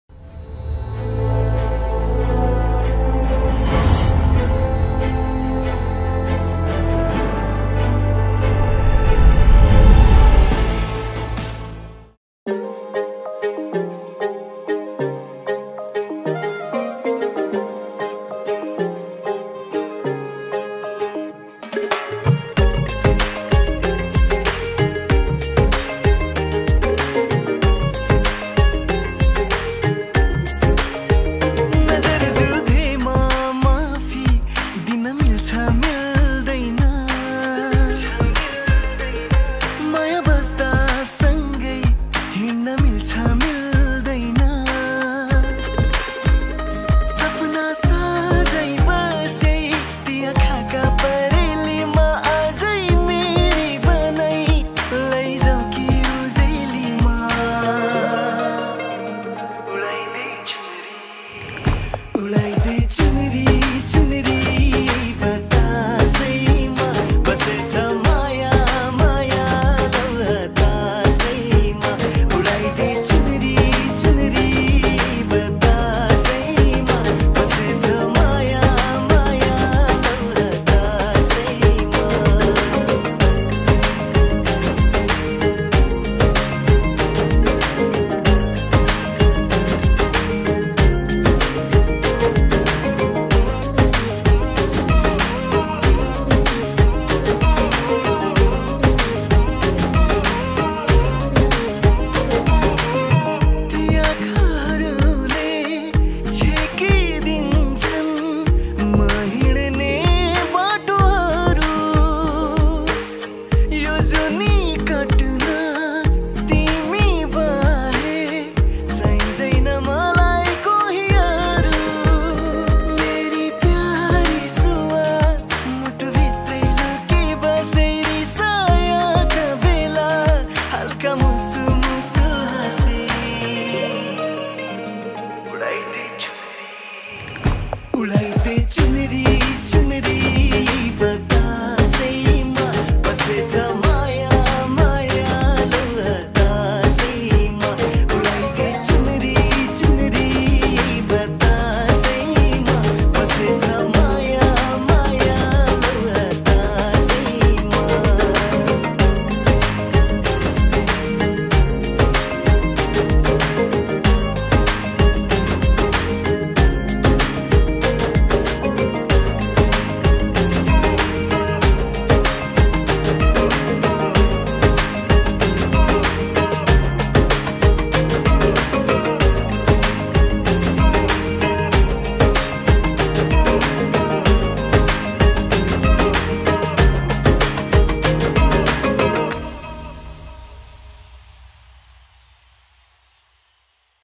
Nepali Cinematic Engagement Highlight 2023//Dr. Binaya & Dr.Prashamsa//Reliable Wedding//Cover Video
Remember us for any kind of Programs, Birthday, Engagement, Wedding, etc for Photography...📷🎥🎬 or Video Services.: ☎️: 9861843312, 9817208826

Singer | Lyrics | Music
Santosh Sunar

Music Produced/ Mixing/ Mastering
Brijesh Shrestha

© 8K Everest Motion Pictures Pvt. Ltd.